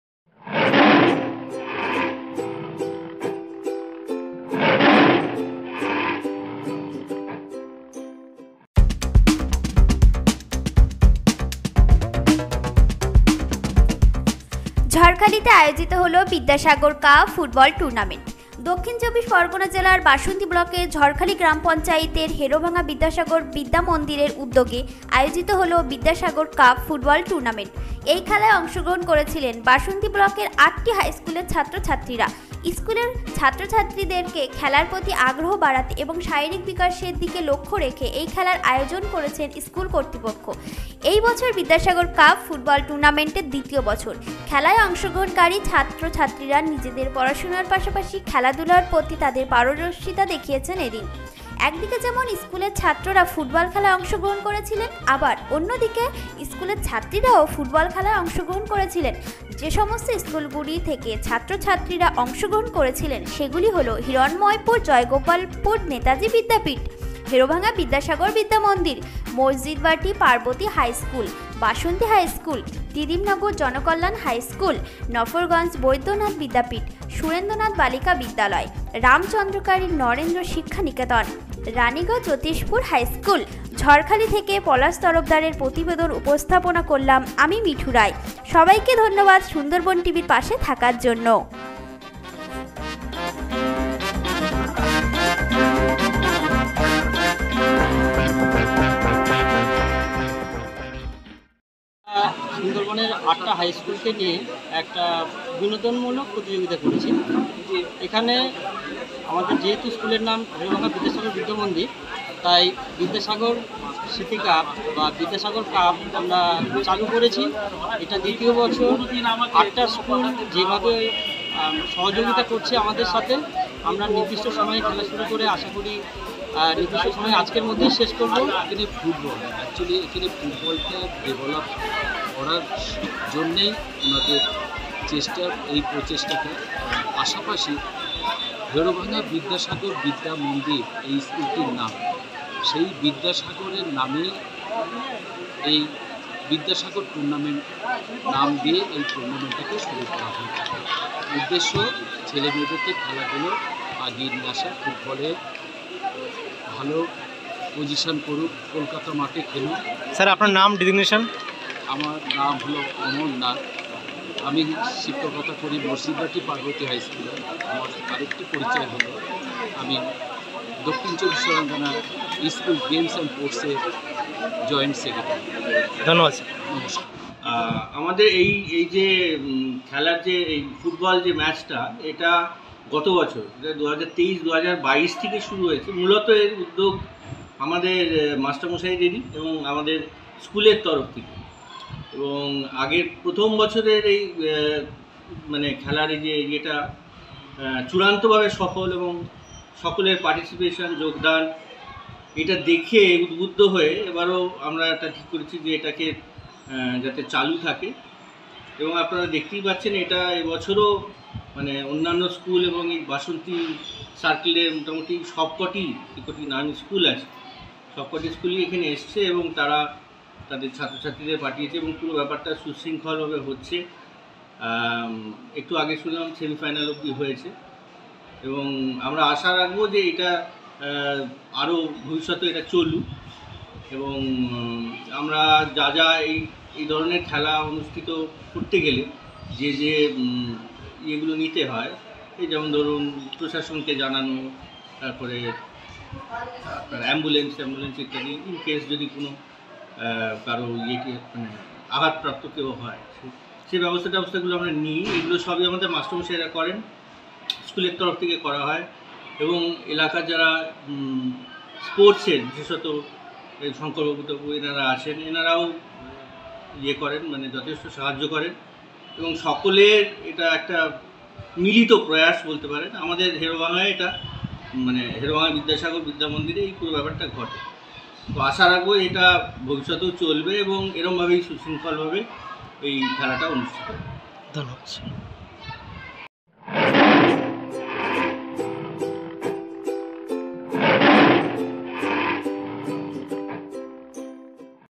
ঝড়খালীতে আয়োজিত হল বিদ্যাসাগর কাপ ফুটবল টুর্নামেন্ট
পলাশ তরফদার ,সুন্দরবন টিভি , ঝড়খালীঃ  দক্ষিণ ২৪ পরগনা জেলার বাসন্তী ব্লকের ঝড়খালি গ্রাম পঞ্চায়েতের হেড়োভাঙ্গা বিদ্যাসাগর বিদ্যামন্দিরের উদ্যোগে আয়োজিত হল বিদ্যাসাগর কাপ ফুটবল টুর্নামেন্ট । এই খেলায় অংশগ্রহণ করেছিলেন বাসন্তী ব্লকের আটটি  স্কুলের ছাত্র-ছাত্রীরা । স্কুলের ছাত্র-ছাত্রীদেরকে খেলার প্রতি  আগ্রহ বাড়াতে এবং শারীরিক বিকাশের লক্ষ্যে এই খেলার আয়োজন করেছেন স্কুল কতৃপক্ষ । এই বছর বিদ্যাসাগর কাপ ফুটবল টুর্নামেন্টের দ্বিতীয় বছর । খেলায় অংশগ্রহণকারী ছাত্রছাত্রীরা নিজেদের পড়াশোনার পাশাপাশি খেলাধুলার প্রতি তাদের  পারদর্শিতা  দেখিয়েছেন এদিন  । একদিকে যেমন স্কুলের ছাত্ররা ফুটবল খেলায় অংশগ্রহণ করেছিলেন আবার অন্যদিকে স্কুলের ছাত্রীরা ও ফুটবল খেলায় অংশগ্রহণ করেছিলেন । যে সমস্ত স্কুলগুলি থেকে ছাত্র ছত্রীরা অংশগ্রহণ করেছিলেন - হিরণ্ময়পুর জয়গোপালপুর নেতাজী বিদ্যাপীঠ , হেড়োভাঙ্গা বিদ্যাসাগর বিদ্যামন্দির , মসজীদবাটি পার্বতী হাই স্কুল , বাসন্তী হাই স্কুল , ত্রিদীবনগর জনকল্যান হাই স্কুল ,নফরঞ্জ বৈদ্যনাথ বিদ্যাপীঠ , সুরেন্দ্রনাথ বালিকা বিদ্যালয়, রাম চন্দ্রখালী নরেন্দ্র শিক্ষা নিকেতন , রাণীগর জ্যোতিষপুর হাই স্কুল । এদিন এই খেলার বিষয়ে দক্ষিণ ২৪ পরগনা জেলার স্কুল  গেমস এন্ড স্পোর্টসের জয়েন্ট সেক্রেটারি ও মসজীদবাটি পার্বতী হাই স্কুলের শিক্ষক অমরনাথ বলেন “এই খেলার মাধ্যমে স্কুলের ছাত্রছাত্রীরা তারা নিজেদের পড়াশোনার পাশাপাশি খেলার প্রতি তাদের যে দক্ষতা আছে সেটি প্রকাশ ঘটবে । পাশাপাশি শারীরিক বিকাশে এই খেলার একটি আলাদা গুরুত্ব আছে । ছেলেমেয়েদের খেলার প্রতি ভালোবাসা বৃদ্ধি পাবে । এর ফলে  ছাত্র ছাত্রীদের মোবাইলের গেমের প্রতি অনিহা তৈরি হবে যা আমাদের আগামী ভবিষ্যৎ কে এগিয়ে নিয়ে যেতে সক্ষম হবে “।